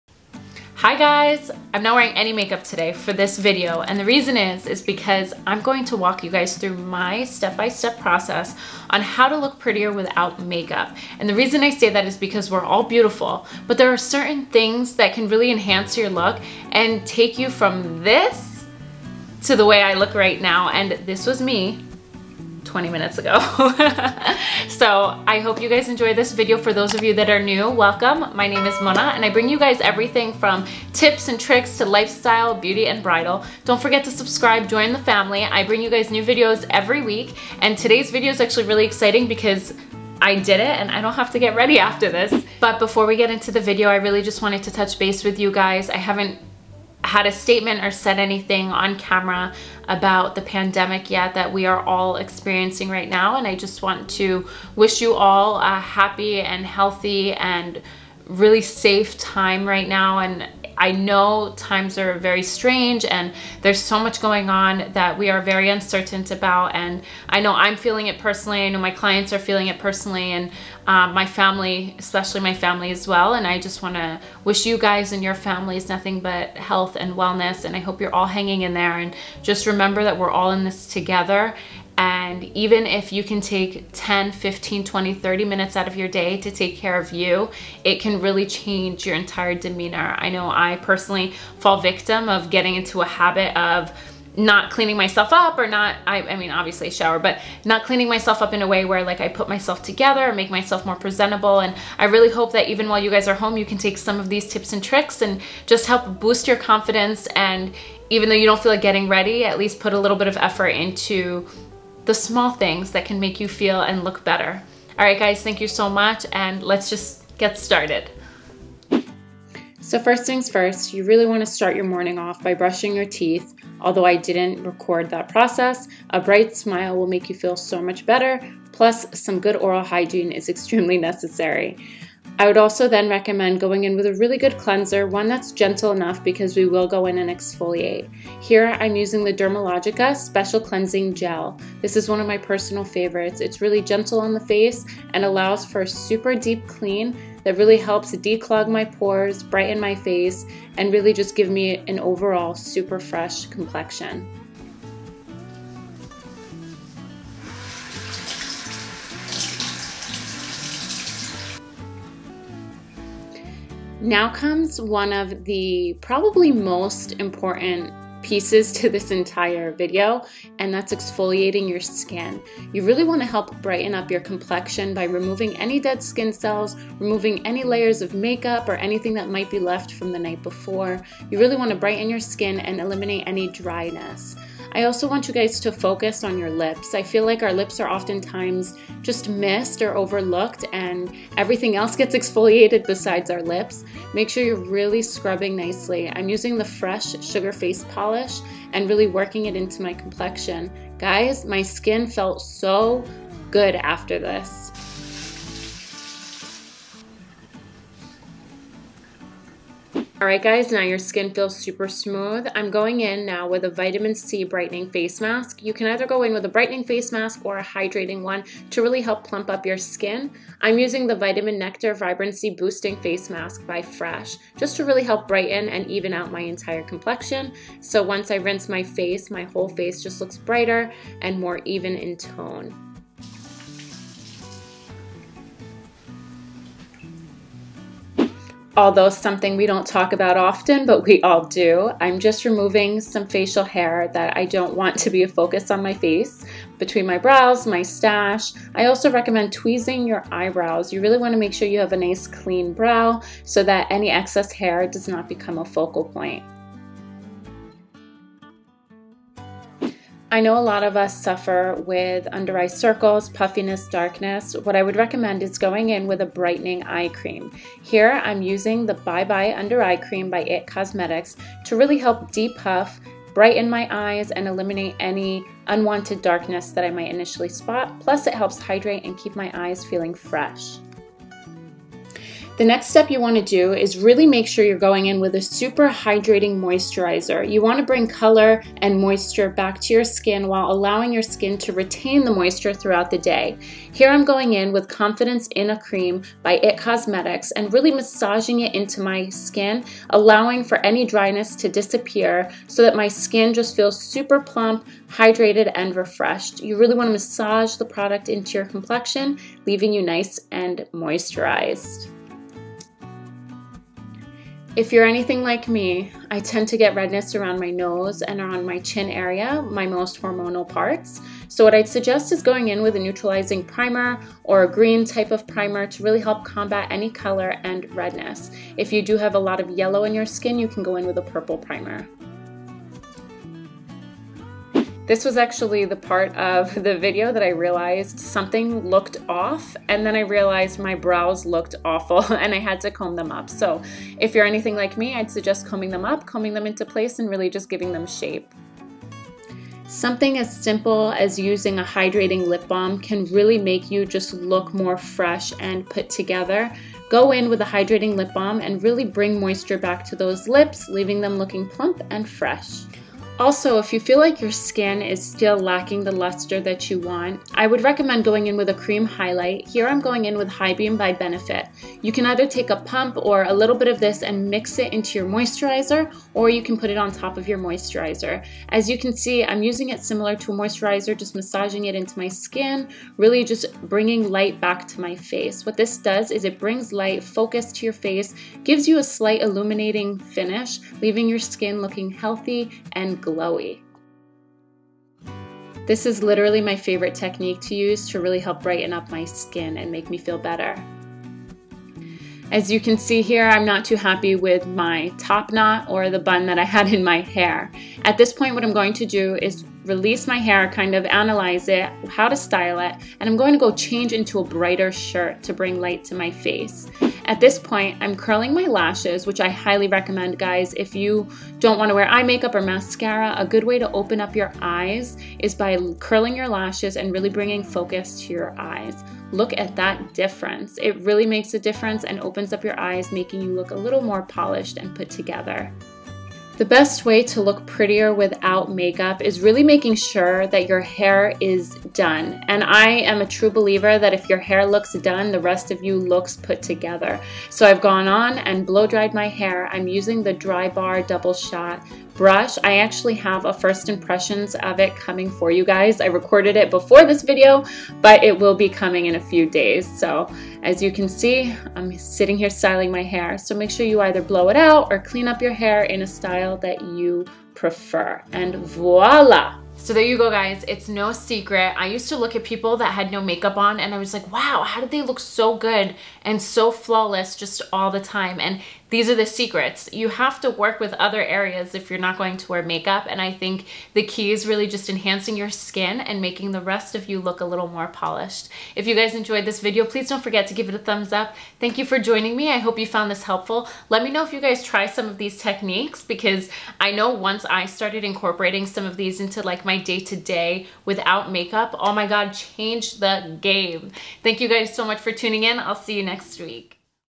QUICK ways to INSTANTLY LOOK PRETTIER without Makeup!
Have you ever thought about different ways you can look prettier without makeup? Looking pretty doesn't have to be complicated! I mean, I am sure, like myself, you don't always feel like putting makeup on! In this video,I show you my quick and easy ways to look prettier without makeup, while enhancing your natural beauty!

PRODUCT DETAILS:

Dermalogica Cleansing Gel

Fresh Sugar Face Polish

Fresh Vitamin Nectar Face Mask

Facial Razors

It Cosmetics Eye Cream

It Cosmetics Confidence In A cream

Cover Girl Neutralizing Primer

Benefit Cosmetics High Beam

Drybar Double Shot Blowdryer

CODE: LADYCHEBLI

CODE: LADYCHEBLI